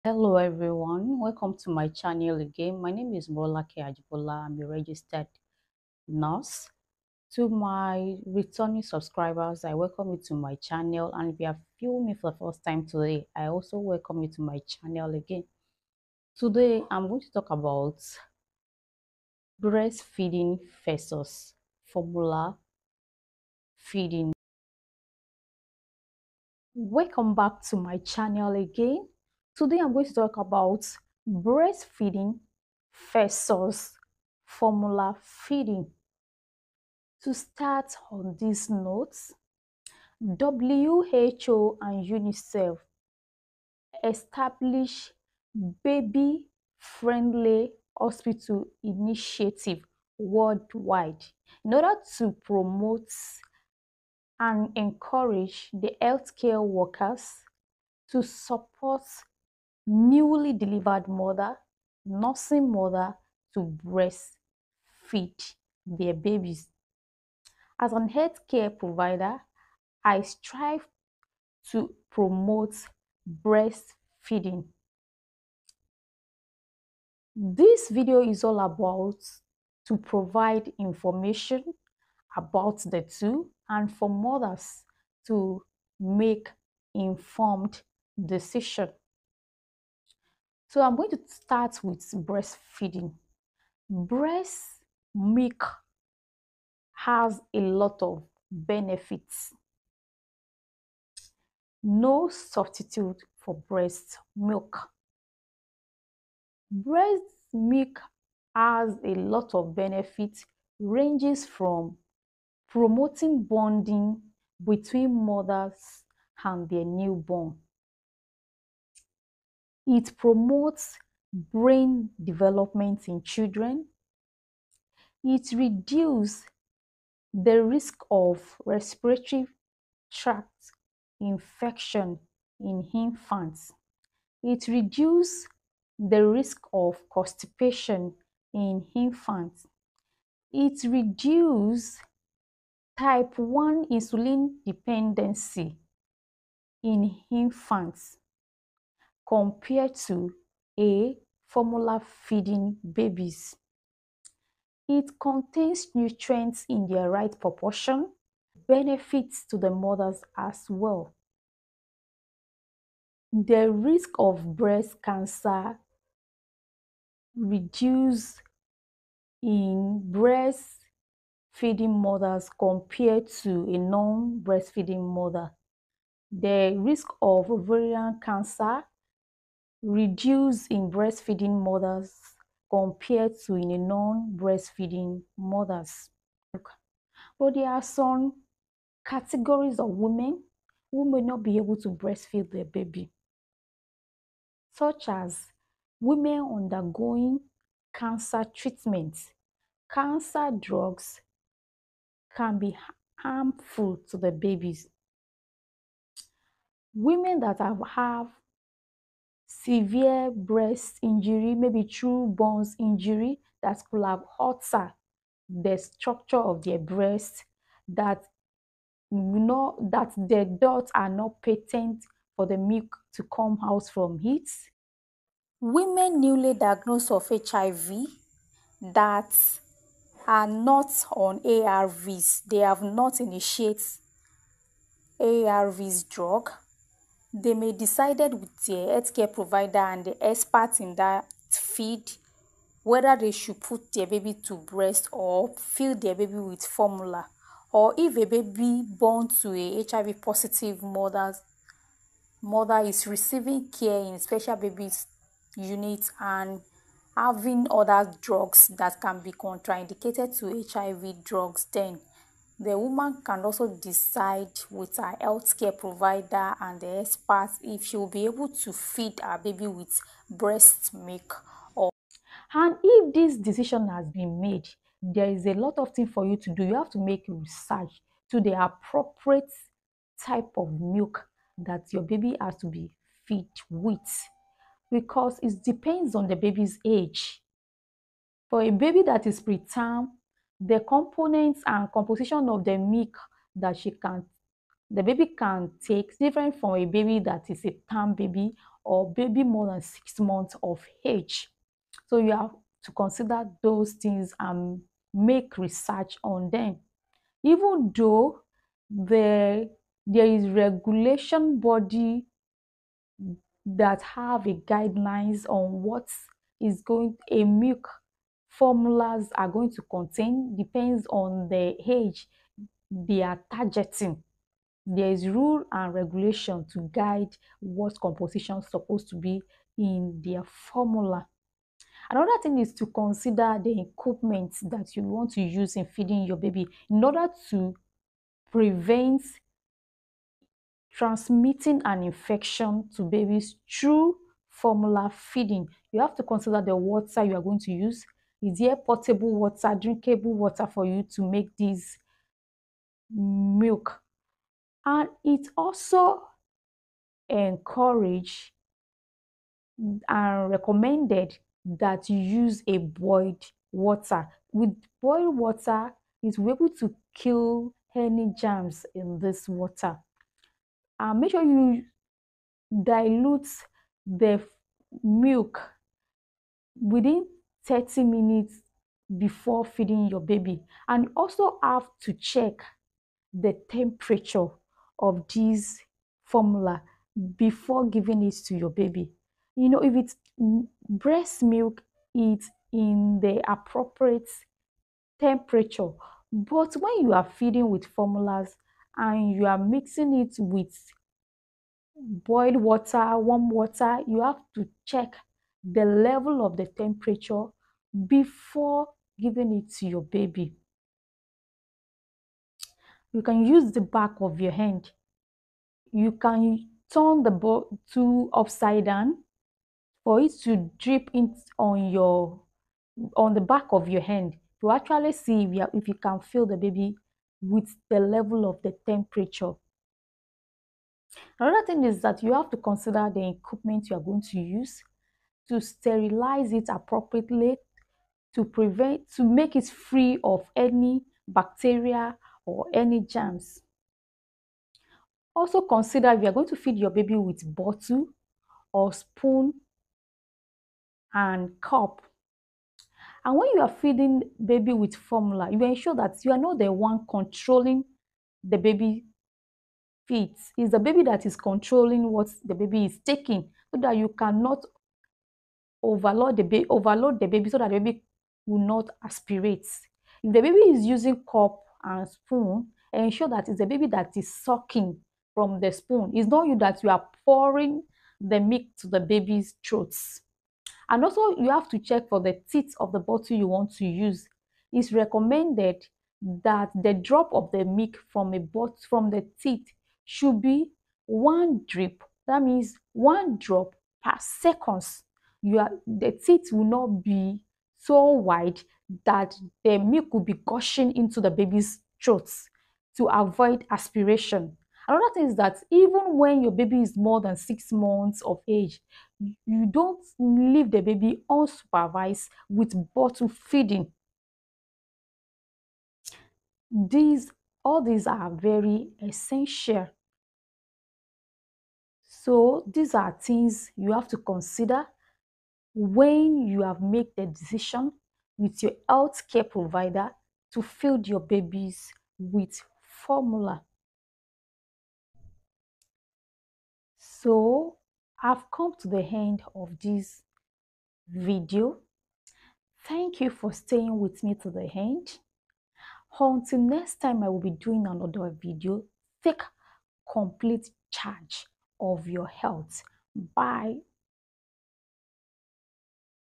Breast milk versus formula feeding infants
This video talks about breast milk versus formula feeding in babies/infants,the benefits, the preparation, categories of women who can not breast feed their newborns.

Breast Milk:
Nutritional Advantages: Breast milk is naturally tailored to a baby's needs, with nutrients that are easily digested and absorbed.
Immune Benefits: Breast milk contains antibodies that help protect babies from infections and illnesses.
Long-Term Health: Breastfed babies may have a lower risk of developing conditions like obesity, diabetes, and asthma later in life.
Bonding: Breastfeeding promotes bonding between mother and child.
Convenience and Cost: Breastfeeding is readily available and free.
Formula:
Nutritional Completeness:
Formula is formulated to provide the essential nutrients needed for infant growth and development.
Convenience:
Formula is easy to prepare and store, making it convenient for parents.
Alternative Feeding Option:
Formula provides a viable option for babies whose mothers cannot or choose not to breastfeed.
Special Needs:
Specialized formulas are available for babies with specific dietary needs or allergies.
Potential Risks:
Formula-fed babies may have a higher risk of certain health problems compared to breastfed babies.

There are categories of women who may not be able to breastfeed their new borns,such as women undergoing cancer treatment, cancer drugs are harmful to babies.

Mothers with burns injury that have altered their breast tissues, breast ducts that can obstruct milk flow/lactation.

Newly diagnosed mothers/pregnant women with HIV and they've not started taken their ARVs drugs or if their babies is in  the SCBU (special baby unit) taken drugs that are contraindicated to ARVs drugs.  These categories of women may discussed with their health care providers or the experts if they can put their babies to breast or introduce formula feeding to their babies.

There are things to consider when women decided to feed their babies with formula.

Do research on suitable choice of formula to introduce to your baby at certain age

Think about the water that you would be using in preparing your baby milk.

Avoid transmitting of bacteria or germ to your babies through formula.

Supervise baby when feeding with bottle.

/@Healthypathways751?sub_confirmation=1